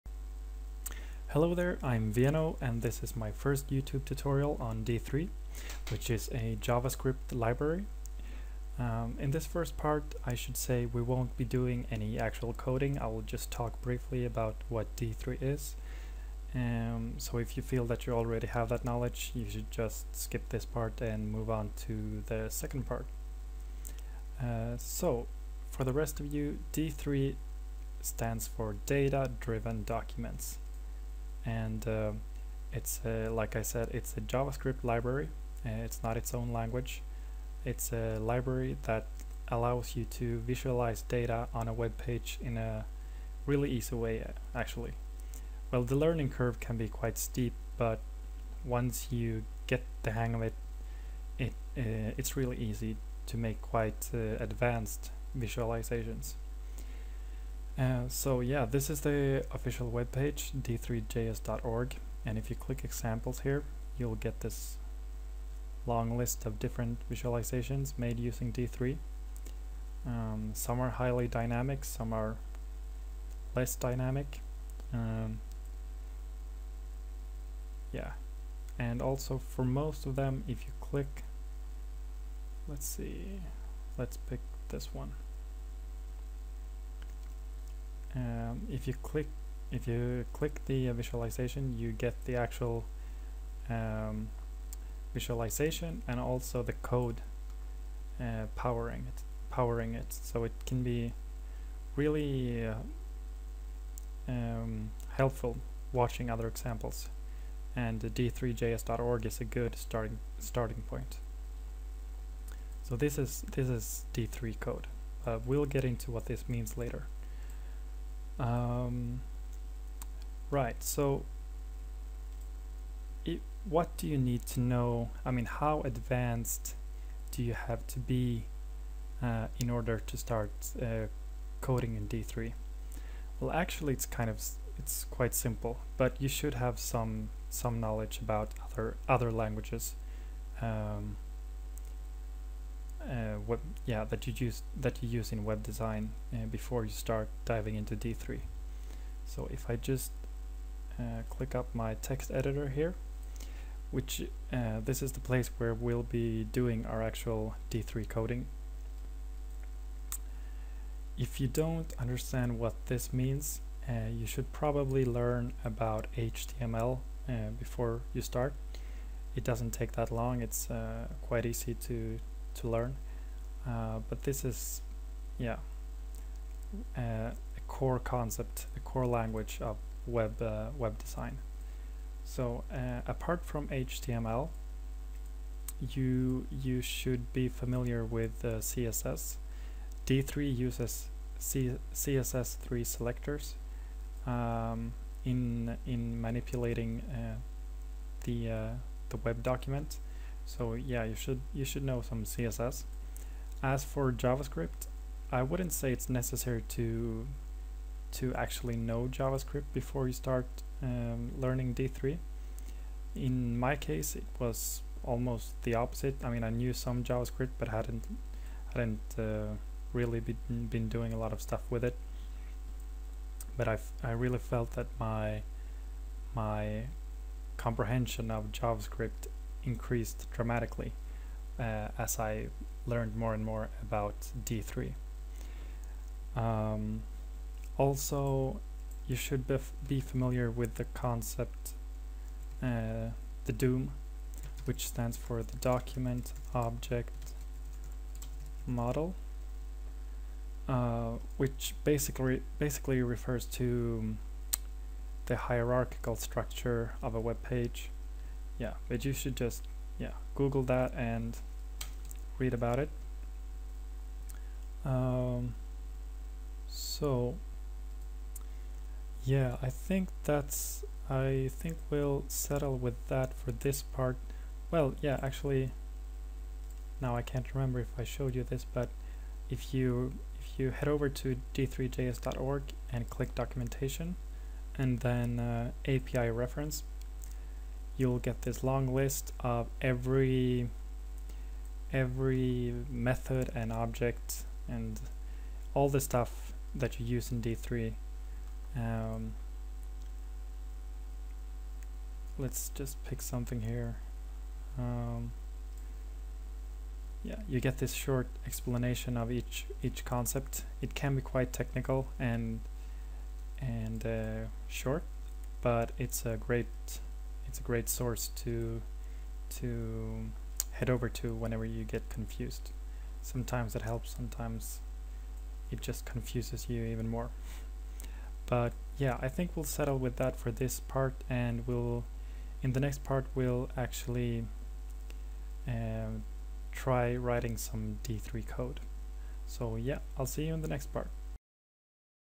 D3.js tutorial - 1 - Introduction
Part 1 of a series of tutorials on the Javascript library D3.